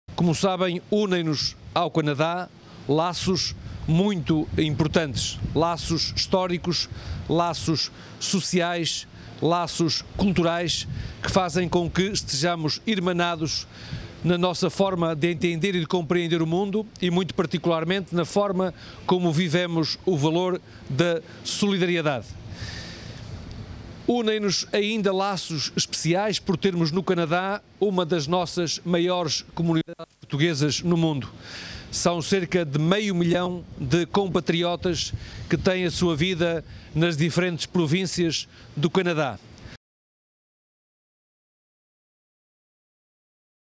Força de 140 operacionais portugueses partiu para o combate aos fogos no Canadá
Os 140 operacionais que constituem a Força Nacional Conjunta que vai ajudar a combater os fogos no Canadá partiu do aeródromo militar de Figo Maduro, em Lisboa, num voo comercial que vai fazer escala em Espanha.